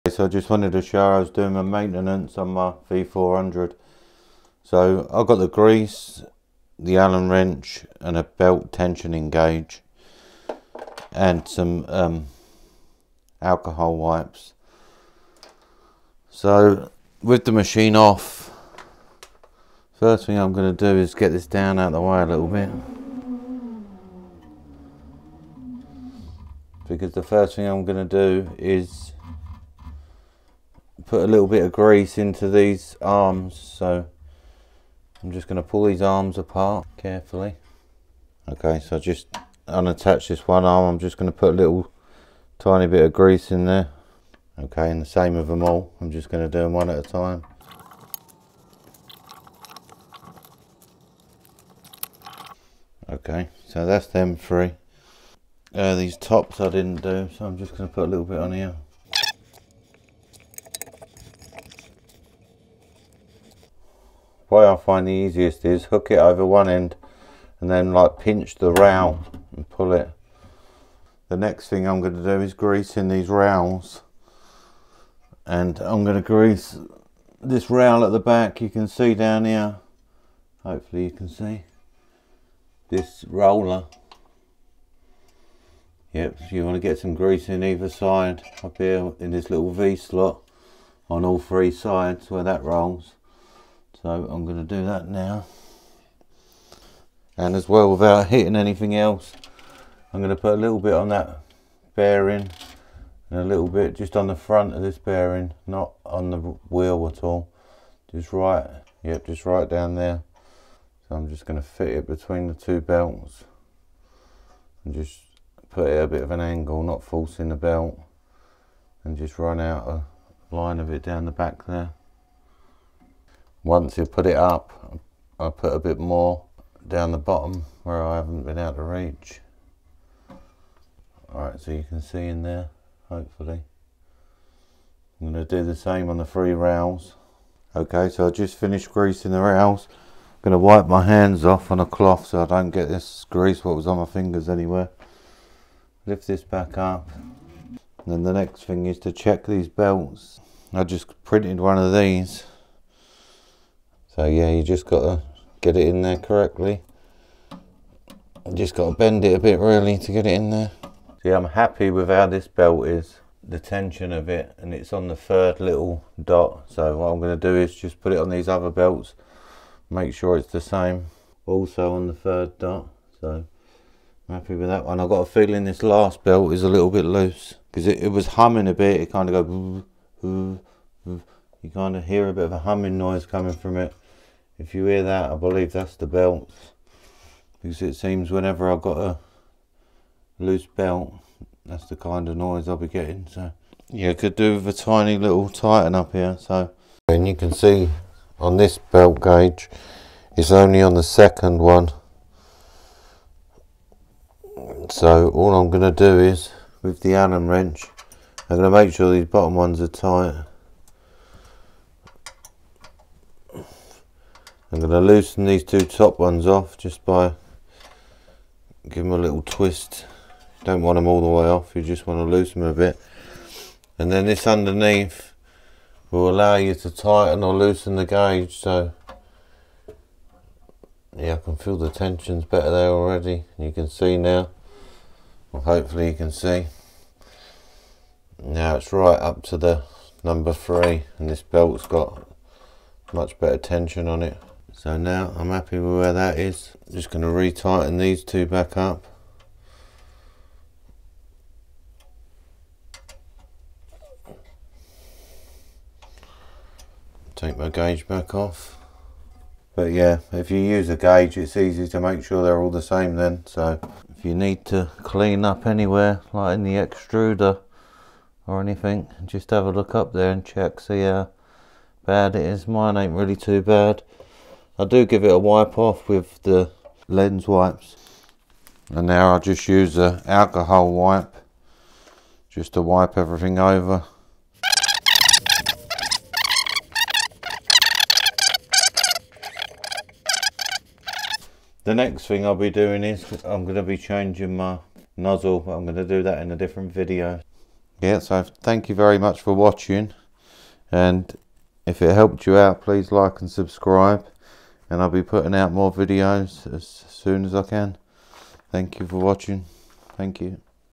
FLSUN V400 REGULAR MAINTENANCE CHECKS
some checks and maintenance for the FLSUN V400

0:00 Intro
0:27 Grease
2:28 Belts
4:22 Check and clean